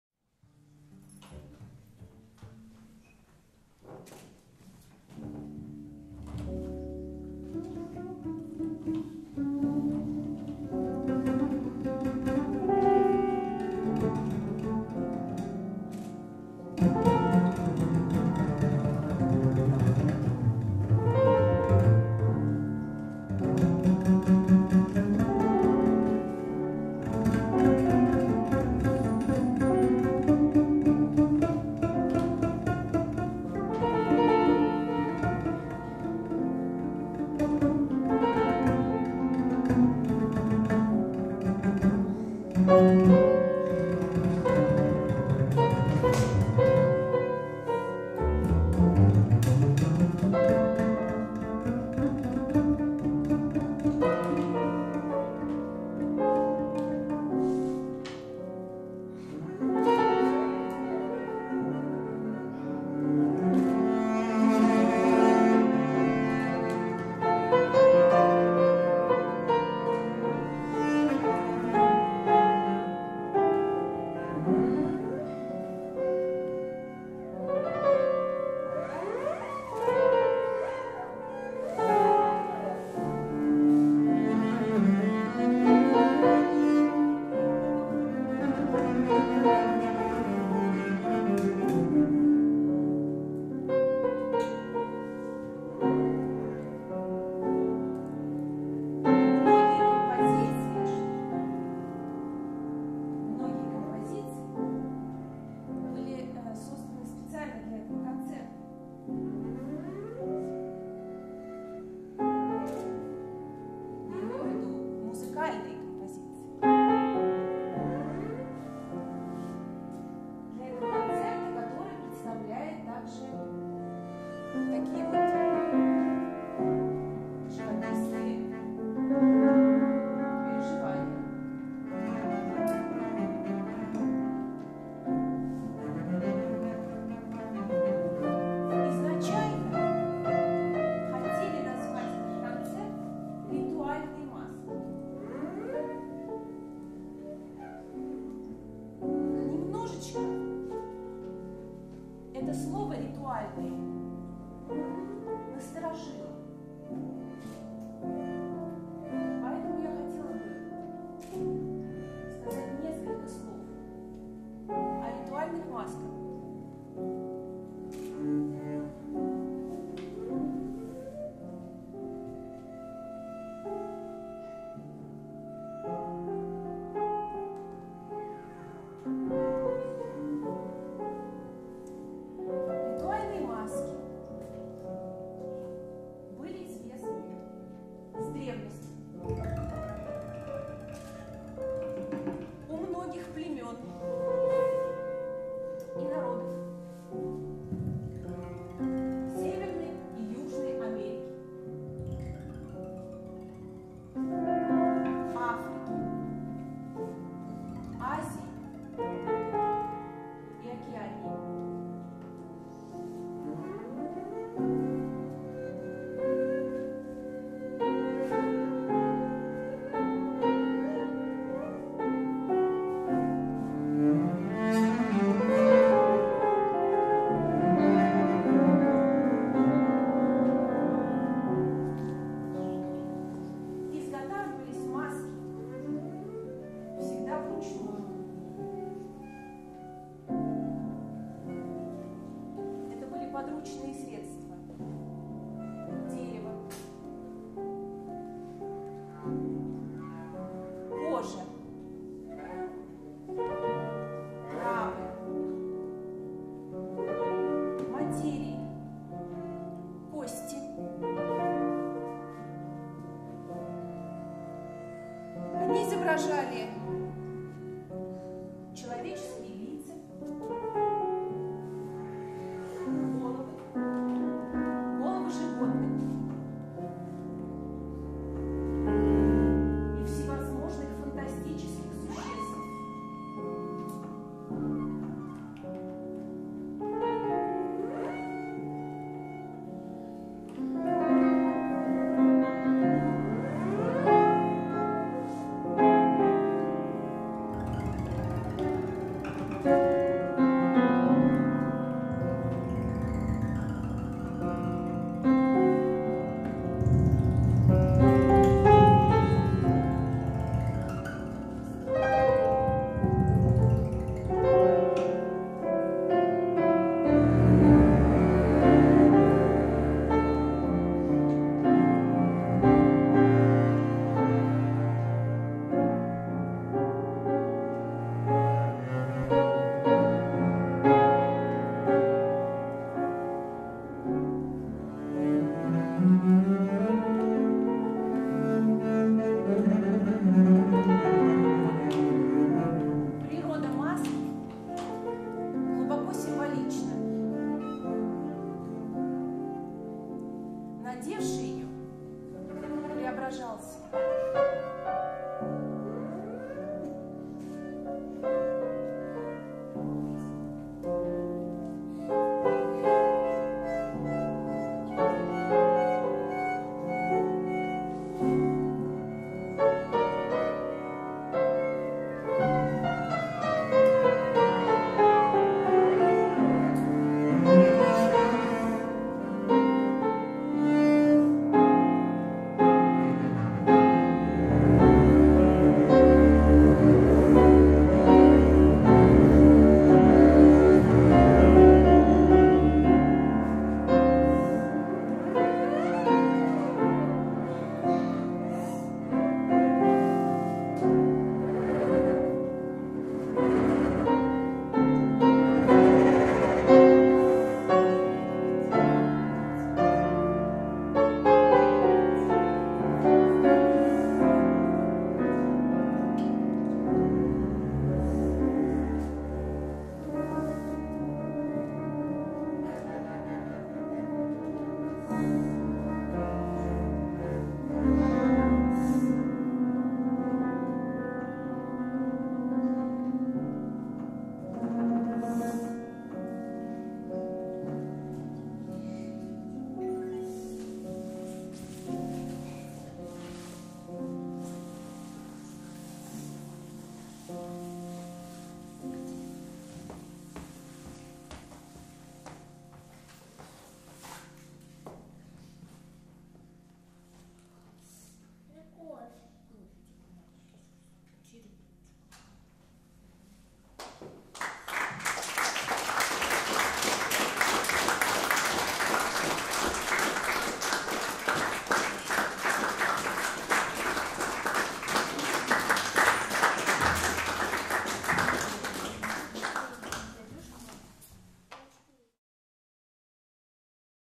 04 "Маски.Отражения"
Концерт «Маски. Отражения» Вадима Бондаренко
Музей Л.Н.Бенуа на Васильевском
Владимир Волков - контрабас, виола да гамба, калимба, кора
Мария Федотова - флейта.
Вадим Бондаренко - музыка, живопись, фортепиано, кларнет.